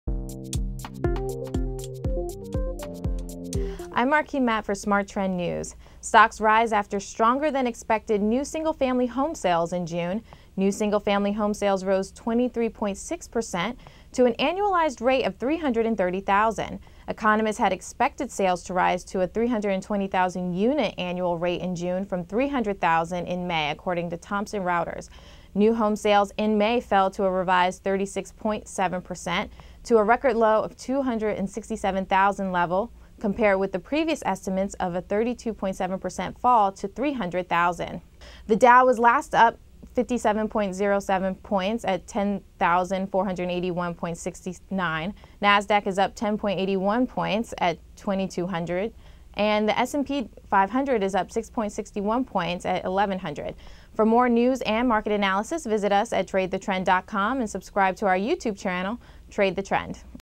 Regular Session: Stocks Gain On Better-than-Expected June New Home Sales Data
10:06 AM, Jul 26, 2010 -- Stocks rise after stronger-than-expected new single-family home sales in June.
New single-family home sales rose 23.6% to an annualized rate of 330,000. Economists had expected sales to rise to a 320,000 unit annual rate in June from 300,000 in May, according to Thomson Reuters. New-home sales in May fell a revised 36.7% to a record low 267,000 level compared with the previous estimate of a 32.7% fall to 300,000.

The Dow was last up 57.07 points at 10,481.69. Nasdaq is up 10.84 points at 2,280.31 and the S&P 500 is up 6.61 points at 1,109.27.